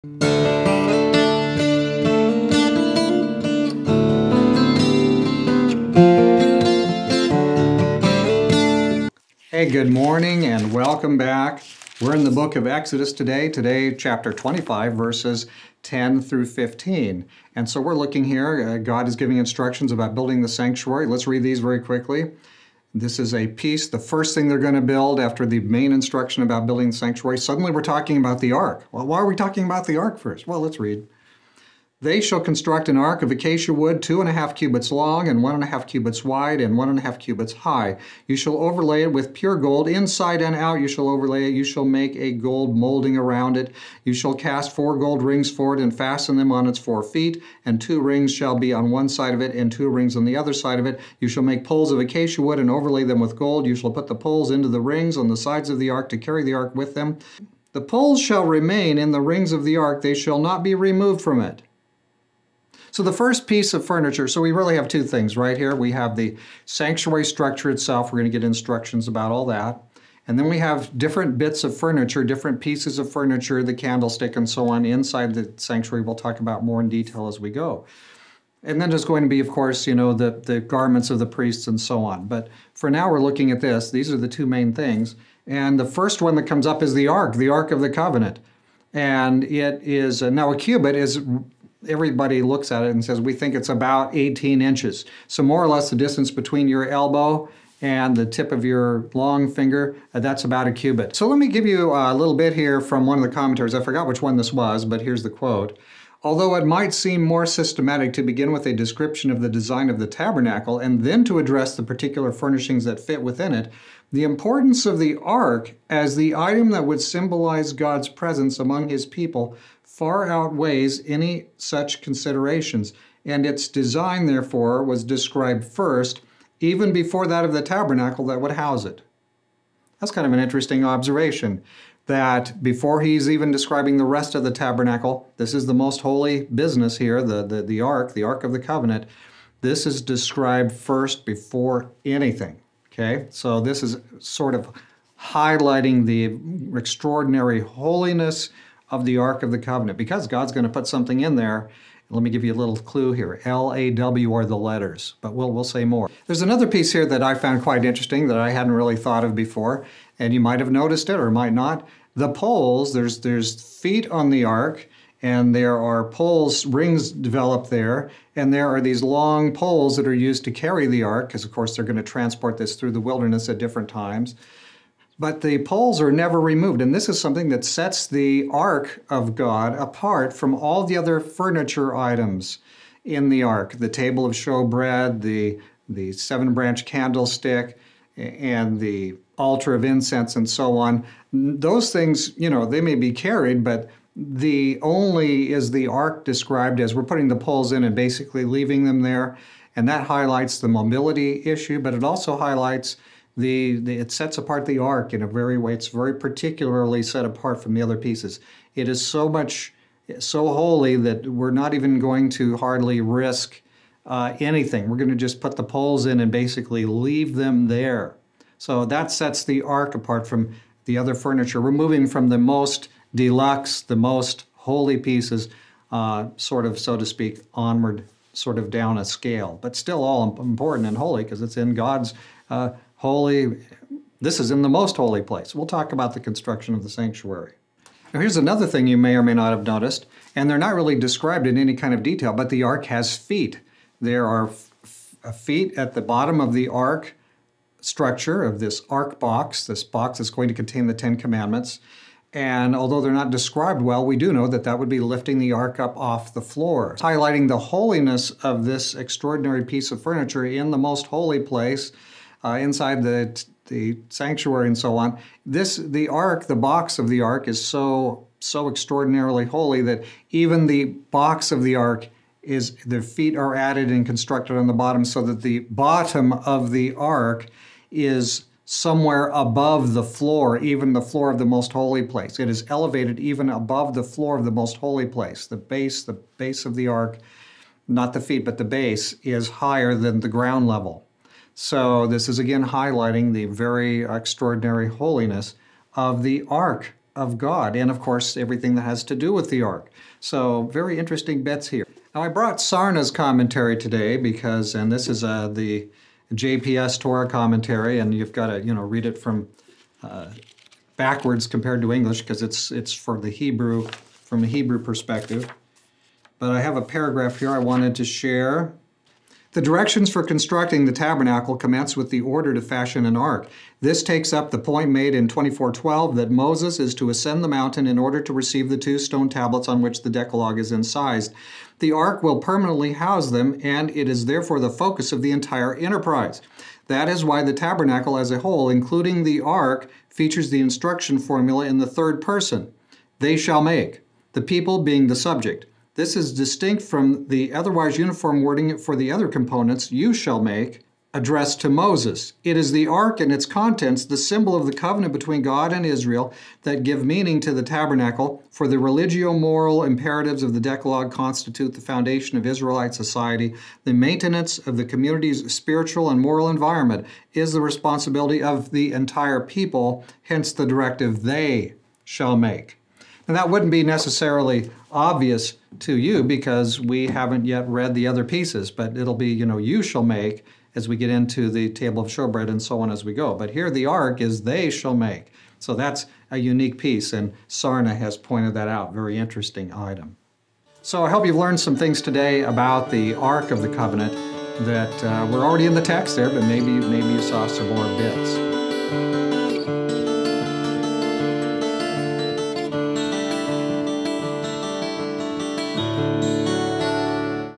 Exodus 25:10-15 Ark of the Covenant
Here are God's instructions about how to construct the ark of the covenant.